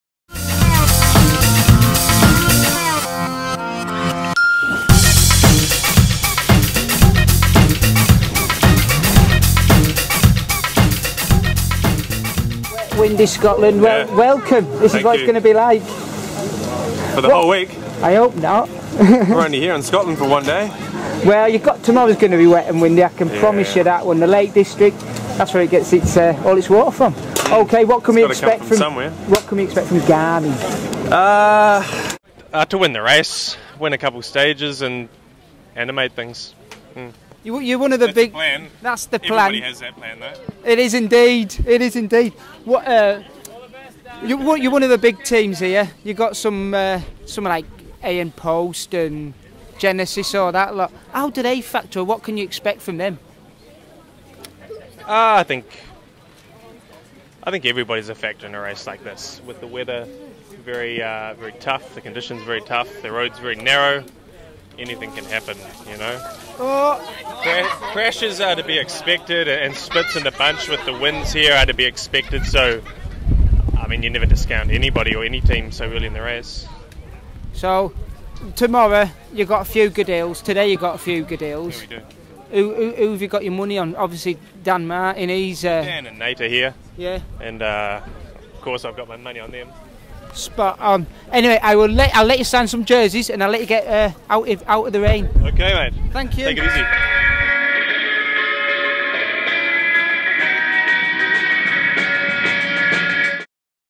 Jack Bauer of Garmin Sharp At The Tour of Britain Stage One
Quick chat with jack Bauer of garmin at the stage start of the firts day at the Tour of Britain 2013. It was wet and very very windy. Not a great day for racing a bike.